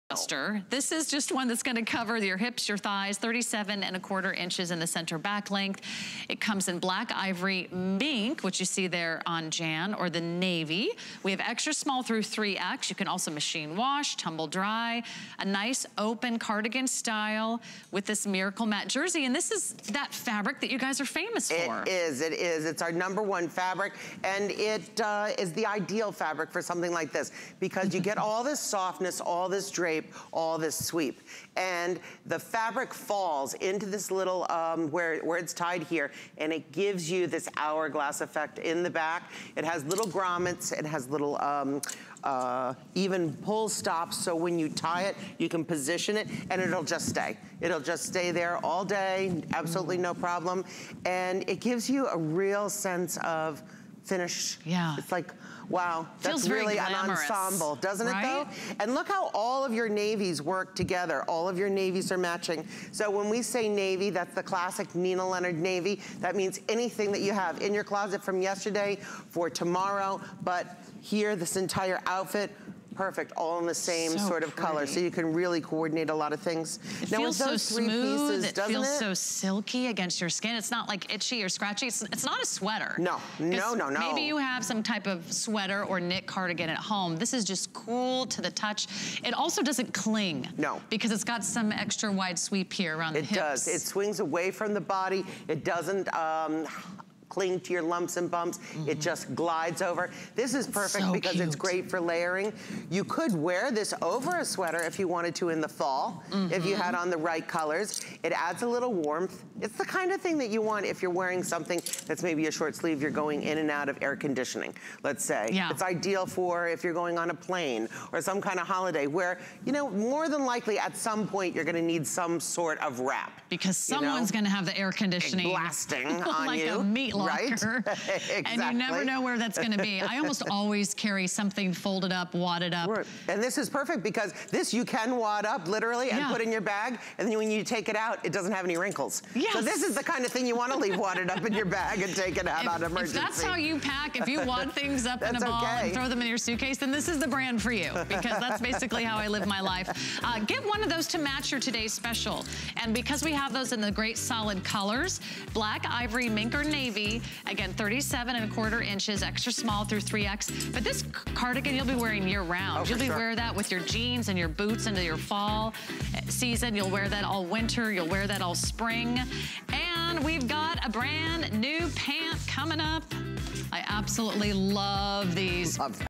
Nina Leonard Miracle Matte Duster Cardigan with Back Tie
Top off sleeveless tops, dresses and tanks with a cardigan. It gives every layered ensemble an added edge and a travelfriendly aesthetic.